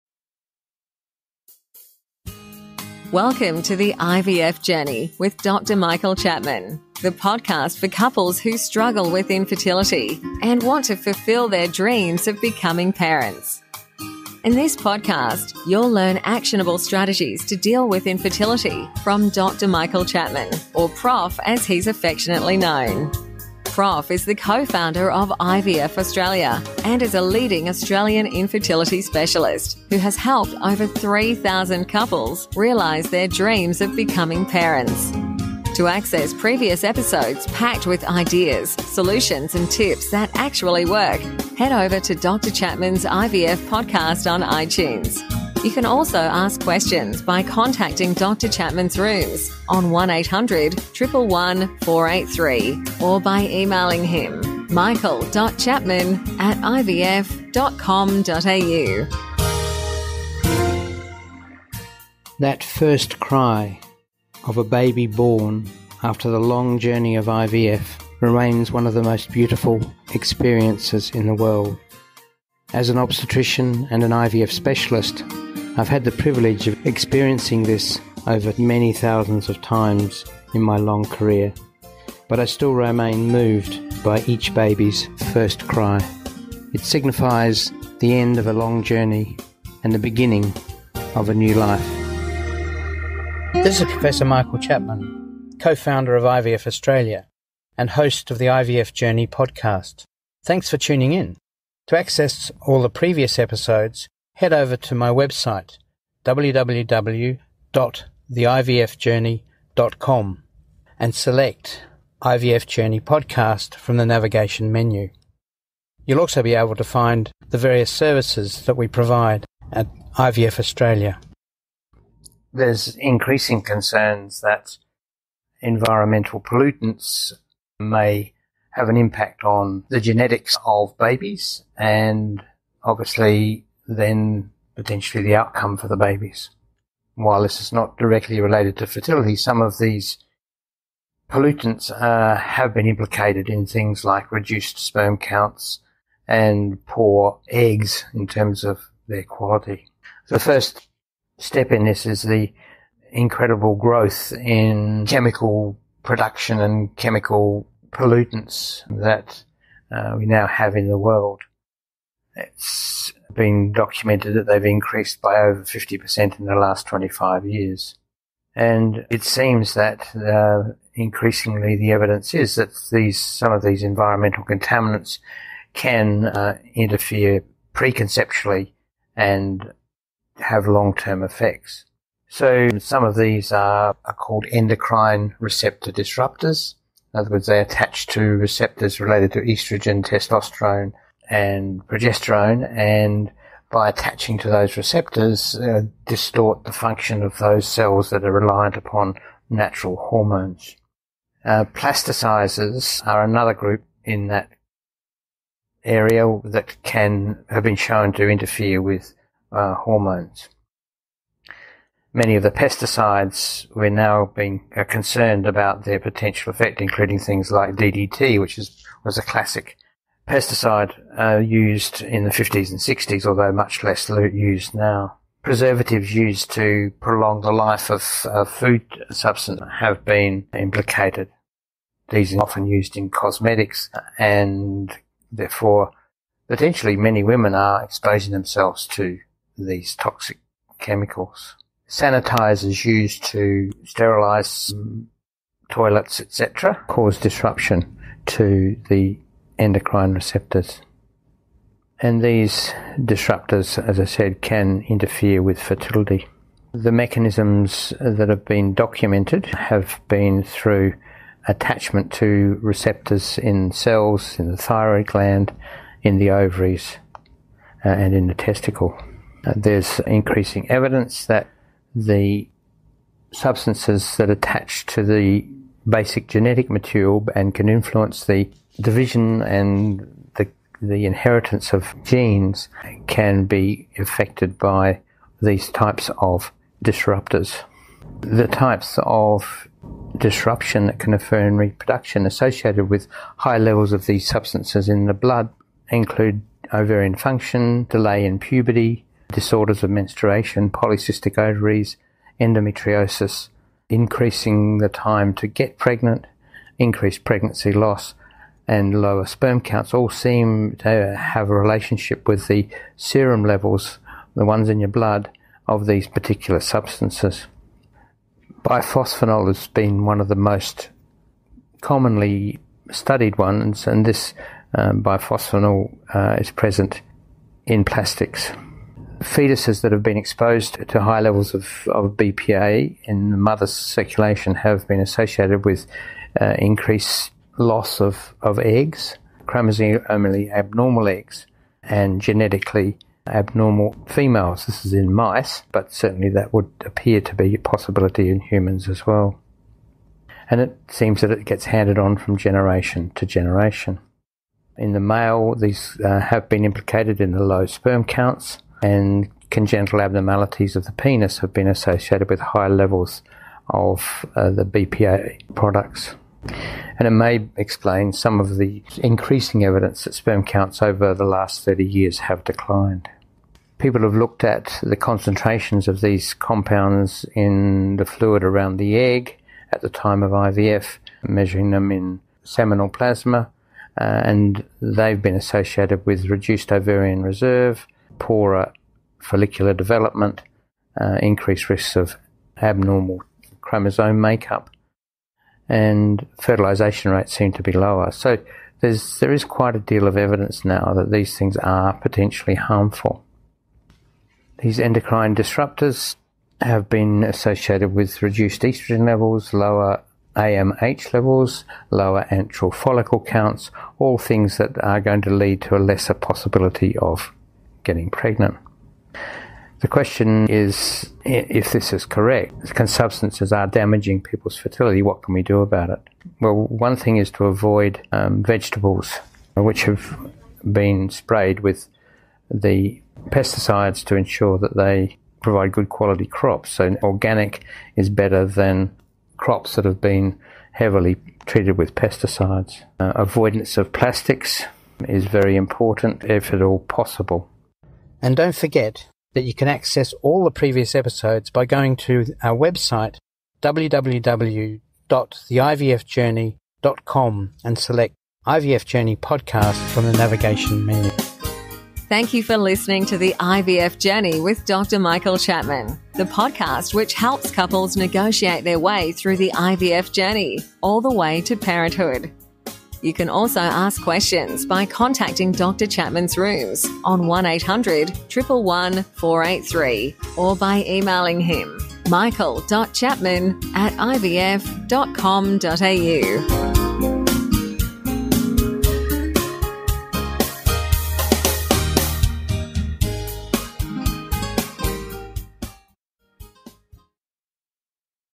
059. Harmful Effects of Pollutants
In today’s episode, we talked about pollutants and its harmful effects.